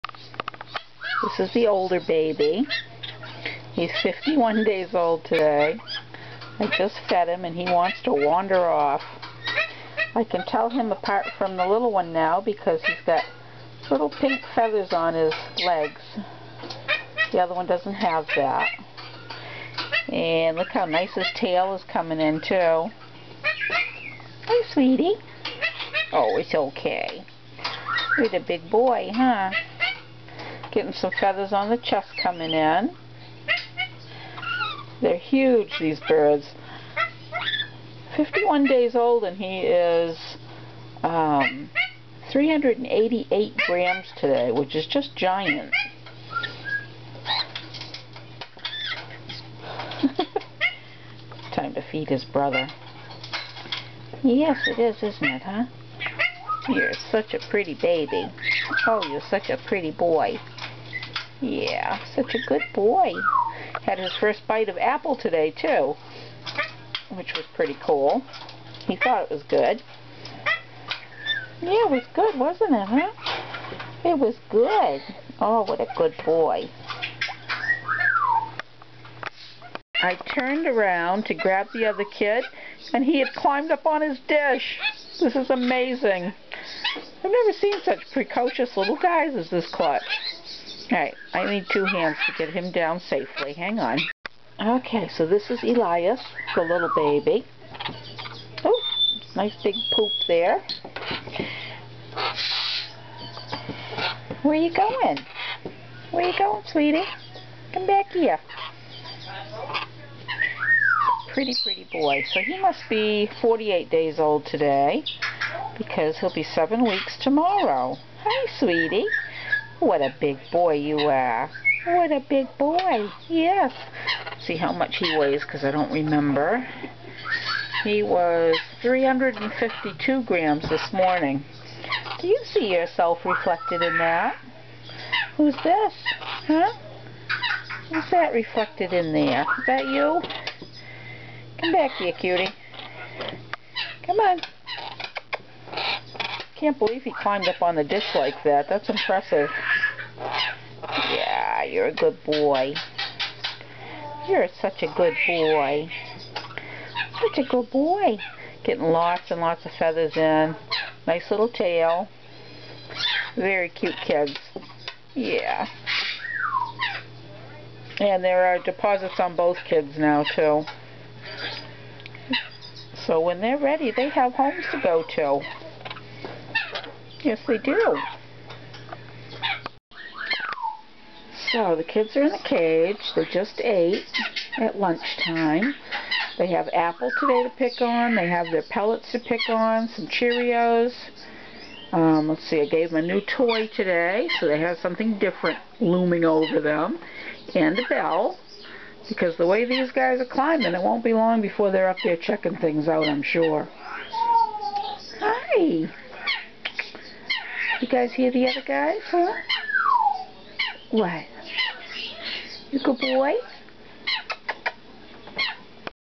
4/4/13 Timneh Chicks 48 and 51 days old
The chicks are now 51 and 48 days old and are named Higgins and Ilyas.  They are very precocious little guys, and very, very cute!  Both have deposits on them, so they are no longer available.